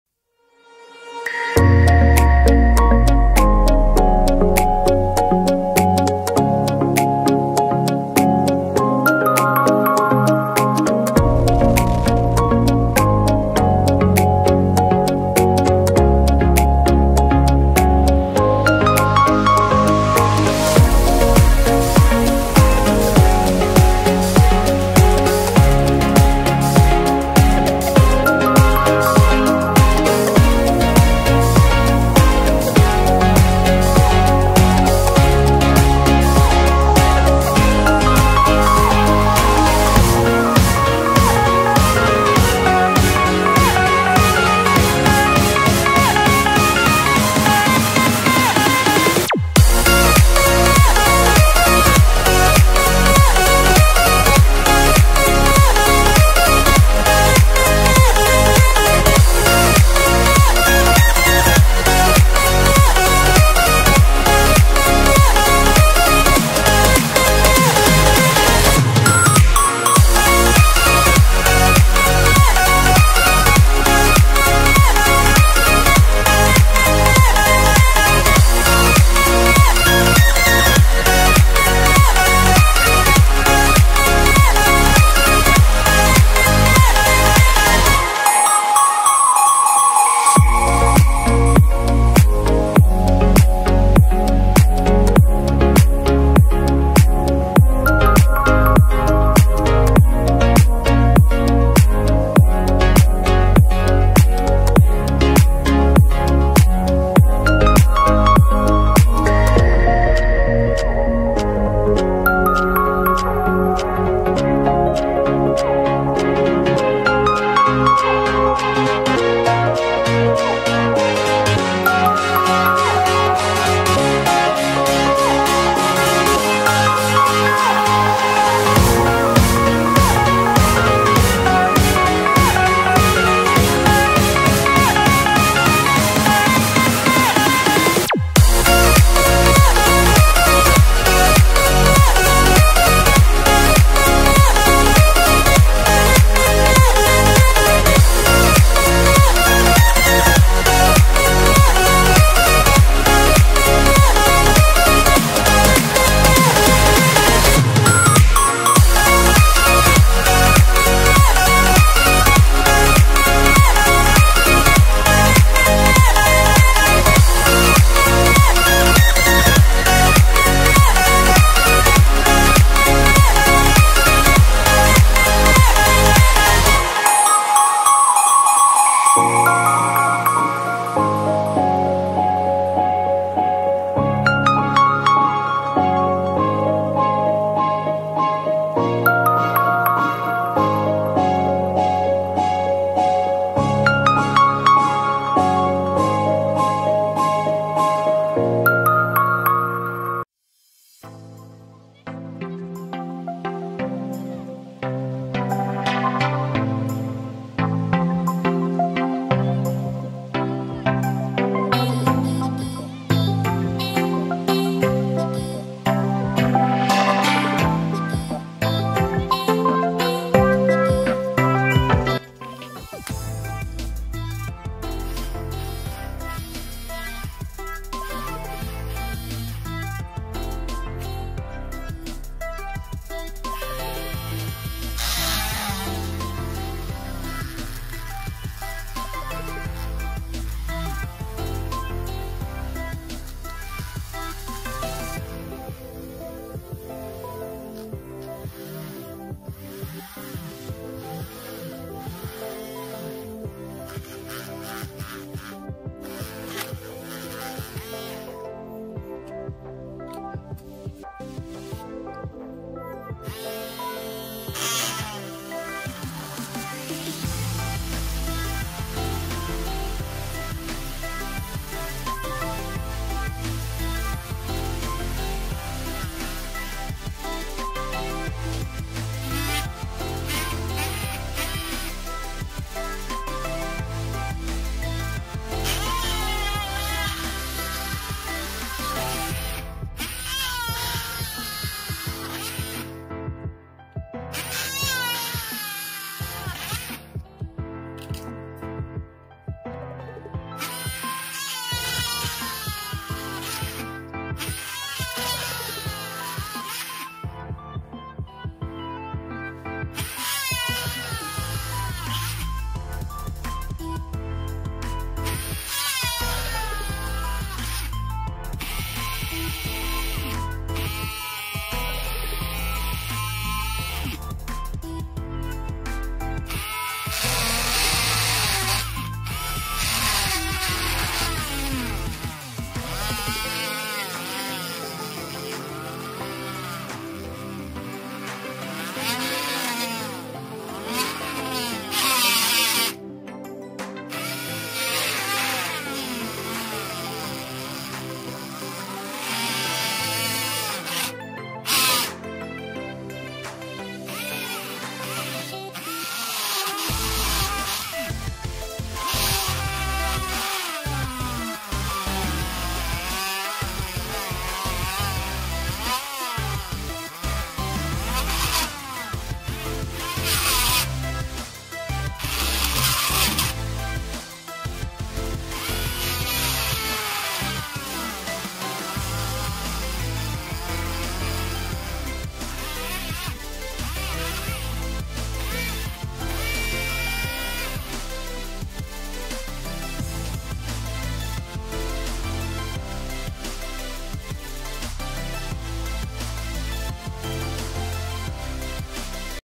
How To Make Drill Machine From Simple DC motor | drill machine kaise banaye
Hi friends welcome to creative things in this video I am going to show you that how to make drill machine from commonly used DC motor, i have used the motor that it mostly available everywhere, and it's very easy to make it so if you like the content then please like share and subscribe thank you for watching this video.